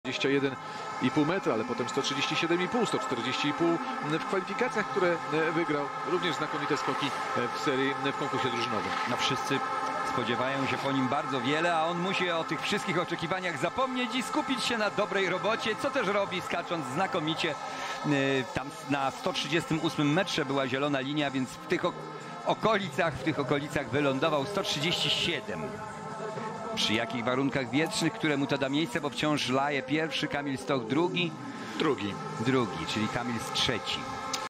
Dawid Kubacki - 137,0m - Zakopane 2020 Seria próbna
Prawa autorskie: TVP